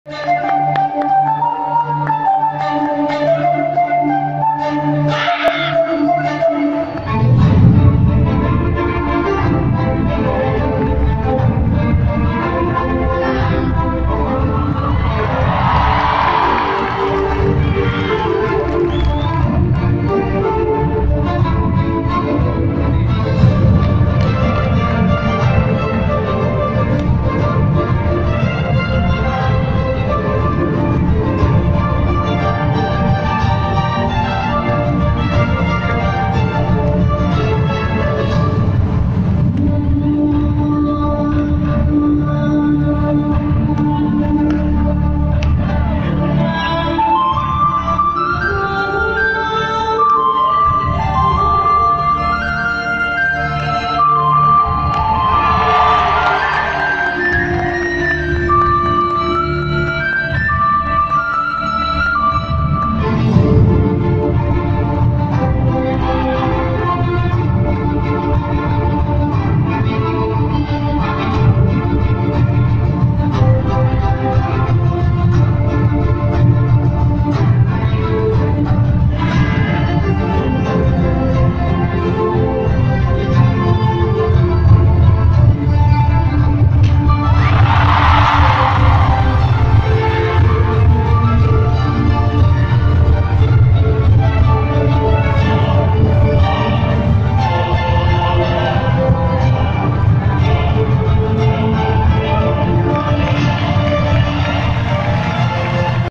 Atlet taekwondo di sponsori Herbalife di Indonesia Spectacular 2018
Pertama sekali tampil di herbalife spectacular 2018